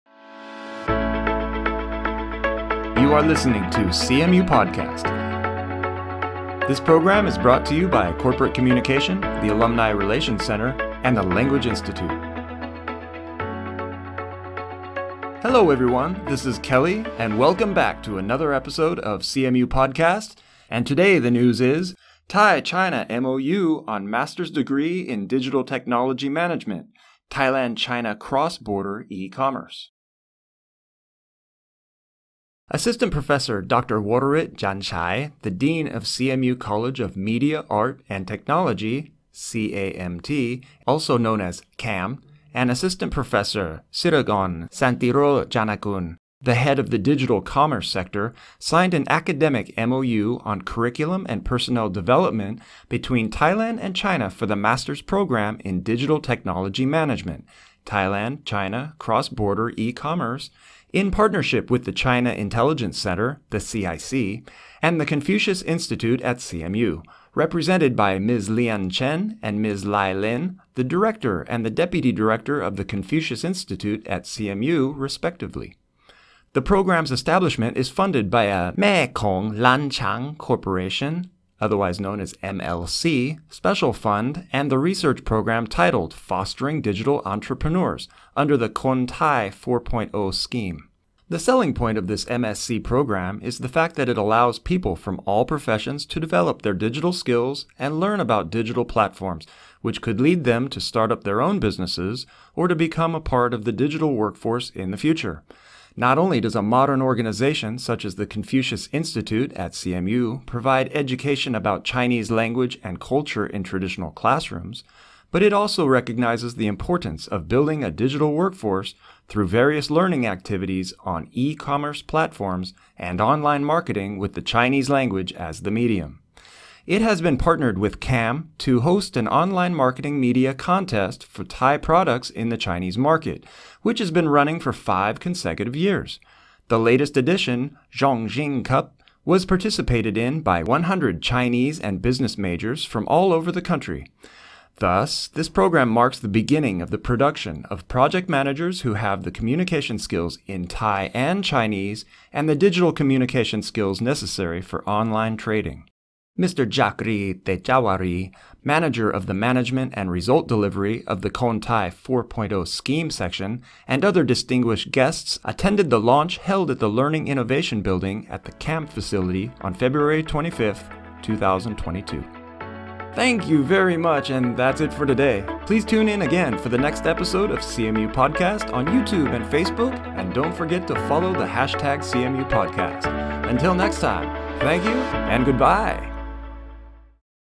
Thai-China MOU on master’s degree in Digital Technology Management
Thai-China MOU on master’s degree in Digital Technology Management (Thailand-China Cross-border E-commerce)

 Assist. Prof. Dr. Worawit Janchai, the Dean of CMU College of Media, Art and Technology (CAMT), and Assist. Prof. Sirikorn Santirojanakul, the Head of the Digital Commerce Sector, signed an academic MOU on curriculum and personnel development between Thailand and China for the Master’s programme in Digital Technology Management (Thailand-China Cross-border E-commerce) in partnership with the China Intelligence Centre (CIC) and the Confucius Institute at CMU, represented by Ms. Lian Chen and Ms. Lai Lin, the Director and the Deputy Director of the Confucius Institute at CMU, respectively. The programme’s establishment is funded by a Mekong-Lancang Cooperation (MLC) Special Fund and the research programme titled ‘Fostering Digital Entrepreneurs’ under the Khon Thai 4.0 scheme.

 The selling point of this MSc programme is the fact that it allows people from all professions to develop their digital skills and learn about digital platforms, which could lead to them to start up their own businesses or to become a part of the digital workforce in the future. Not only does a modern organization such as the Confucius Institute at CMU provide education about Chinese language and culture in traditional classrooms, but it also recognizes the importance of building a digital workforce through various learning activities on e-commerce platforms and online marketing with the Chinese language as the medium. It has been partnered with CAMT to host an online marketing media contest for Thai products in the Chinese market, which has been running for five consecutive years. The latest edition, Zhong Xing Cup, was participated in by 100 Chinese and business majors from all over the country. Thus, this programme marks the beginning of the production of project managers who have the communication skills in Thai and Chinese, and the digital communication skills necessary for online trading.   Mr. Chakri Techawaree, Manager of the Management and Result Delivery of the Khon Thai 4.0 Scheme section, and other distinguished guests attended the launch held at the Learning Innovation Building, at the CAMT faculty on February 25, 2022.